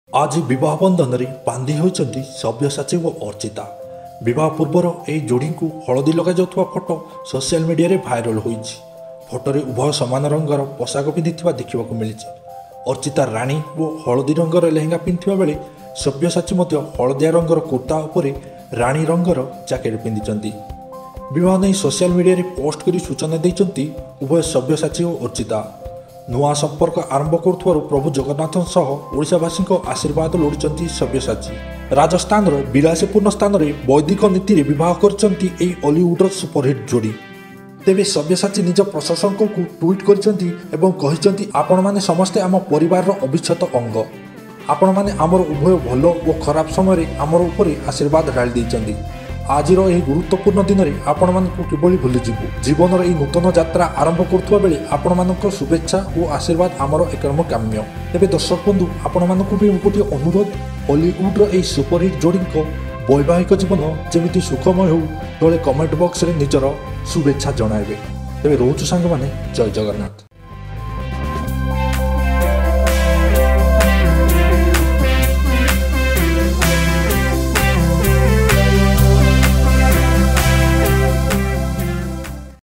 Odia Actor Sabyasachi Mishra Shared Sabya and Archita Marriage New Photo | Weeding Photoshoot Video
Hero Sabyasachi and Archita marriage video album in Rajasthan latest photos both look's beautiful. Ollywood Actors Sabyasachi & Archita Tie Nuptial Knot Today In Real Life.

Note:- All content used is copyright to OdiaPk ,use or commercial display or editing Of the content without proper credit will lead to legal issues against you.